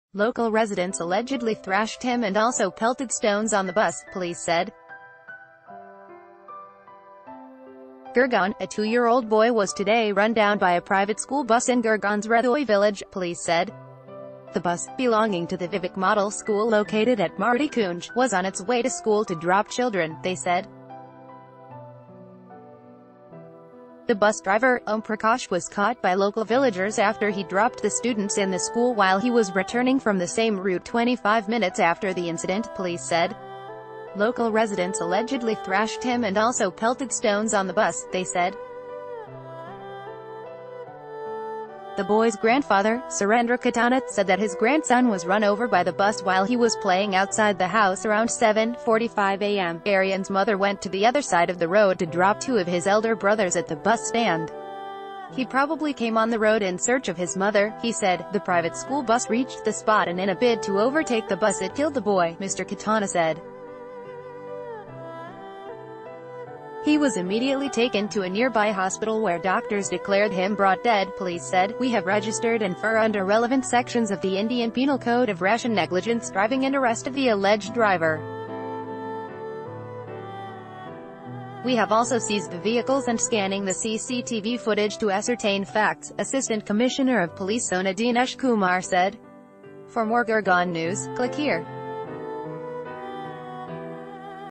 2 Year-Old Run Over By School Bus In Gurgaon
2 Year-Old Run Over By School Bus In Gurgaon